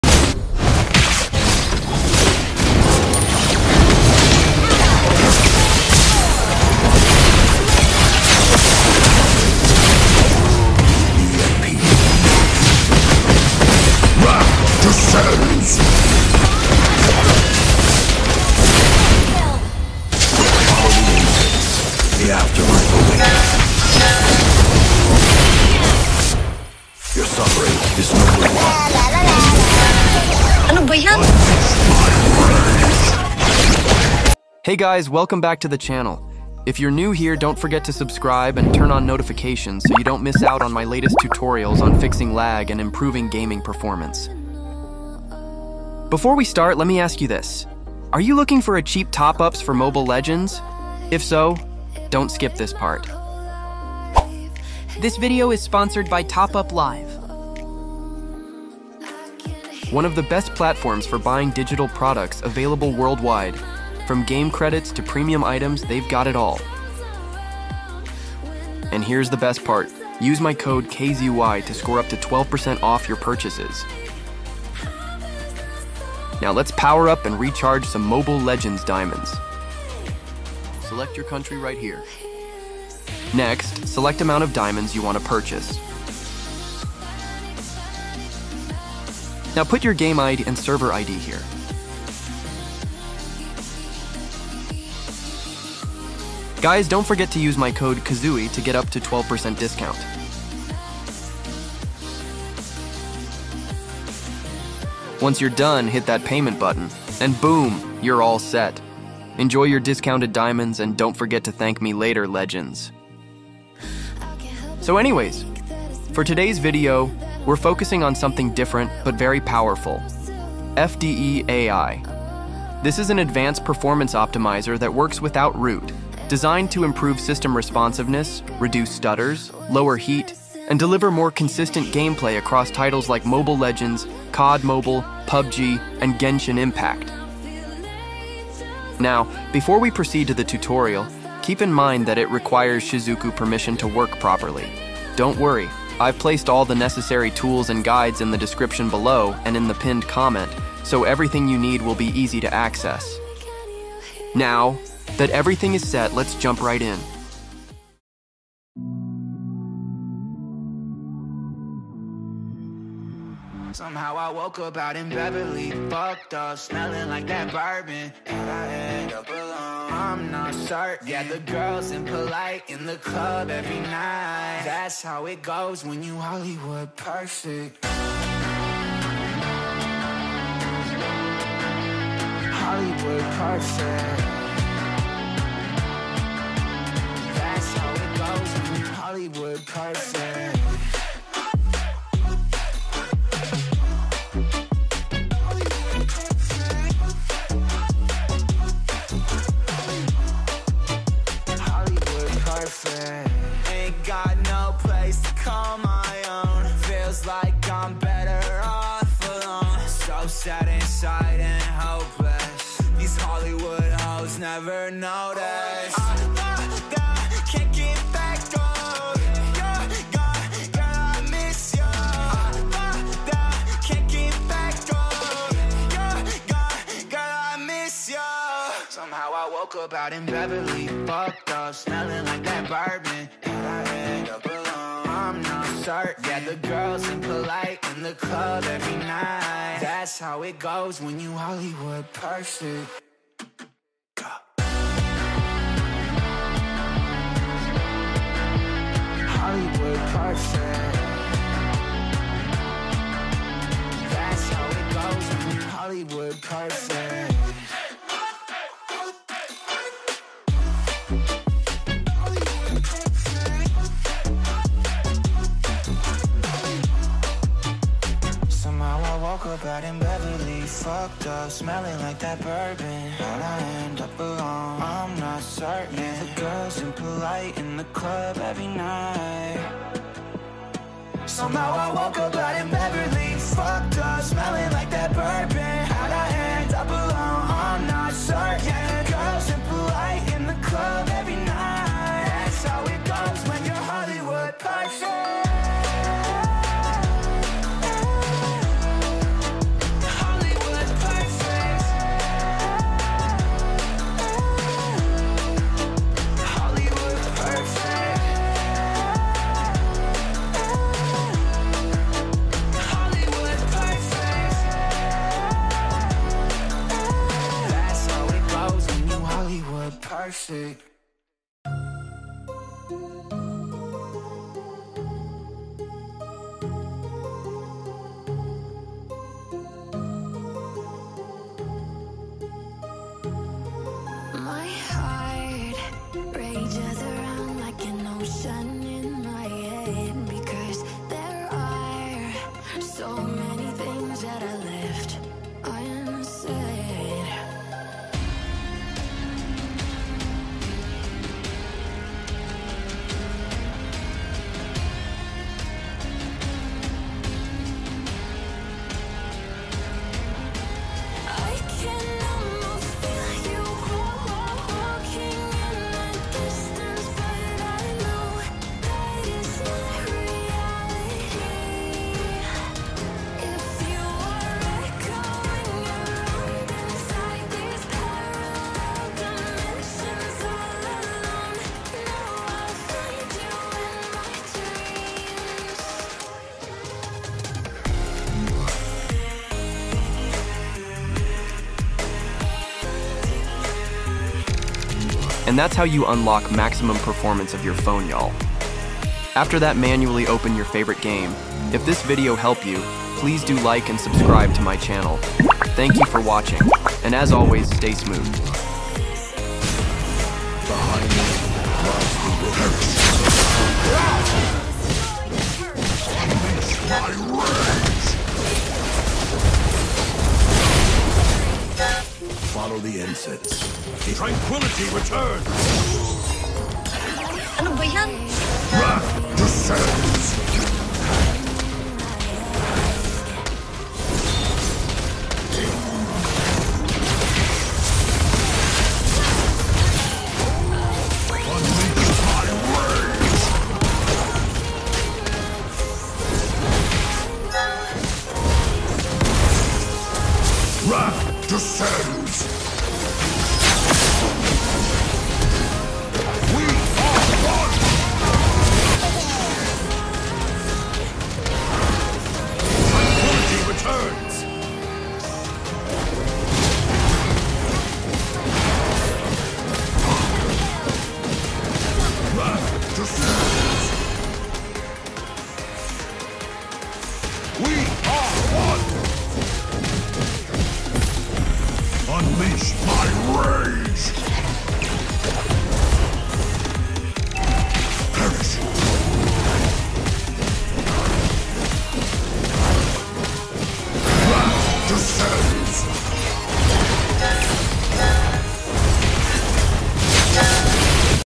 SHIZUKU AI GAME BOOSTER! Turn Laggy Low-end Phone to HIGH RAM Gaming Phone | Fix Fps Drop & Lag ⚡
Want to FIX FPS drops and make your old phone feel like a high-RAM gaming phone? 🎮 In this video, I’ll show you how to optimize your device using Shizuku AI Game Booster—no root required! Perfect for Mobile Legends (MLBB), PUBG, CODM, and other heavy games. Say goodbye to lag, frame drops, and stutters, and enjoy buttery-smooth gameplay even on low-end devices.

⚡ Key Features:
✅ Boost performance with Shizuku AI (no root)
✅ Fix FPS drops, lag, and stutters instantly
✅ Smarter CPU & GPU optimization for gaming
✅ Works on low-end, midrange, and flagship phones
✅ Stable FPS + better battery efficiency
✅ 1-minute easy setup

💸 CHEAP TOP UPS for MLBB, PUBG, CODM

📂 Download & Setup Guide: (Check PINNED COMMENT)

© Credits:
Surface Flinger – Kazuyoo Resources